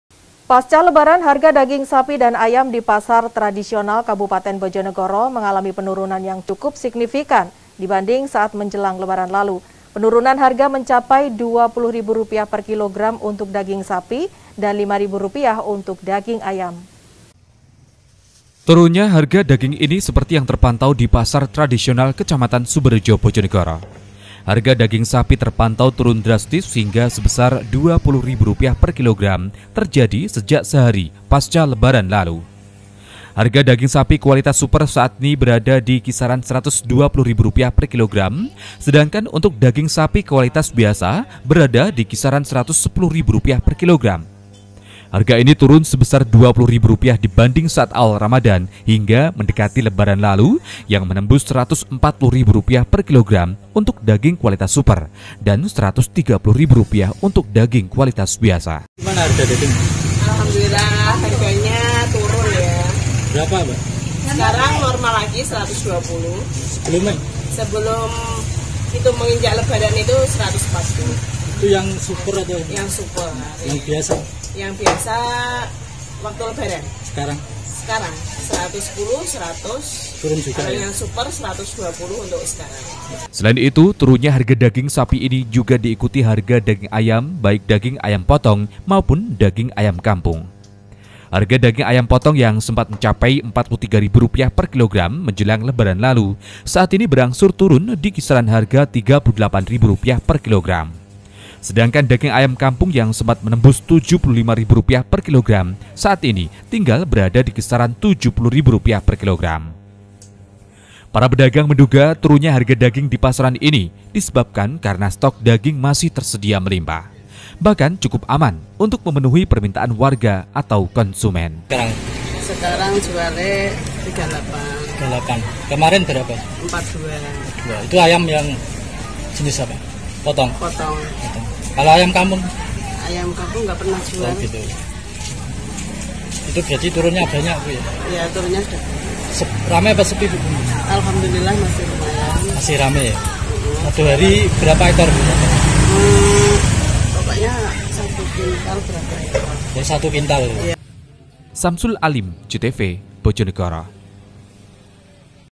BOJONEGORO - Pasca Lebaran, Harga Daging Mulai Menurun
Pasca lebaran harga daging sapi dan ayam di pasar-pasar tradisional kabupaten bojonegoro. Mengalami penurunan yang cukup signifikan dibanding saat menjelang lebaran lalu. Penurunan harga mencapai dua puluh ribu rupiah per kilogram untuk daging sapi dan lima ribu rupiah untuk daging ayam.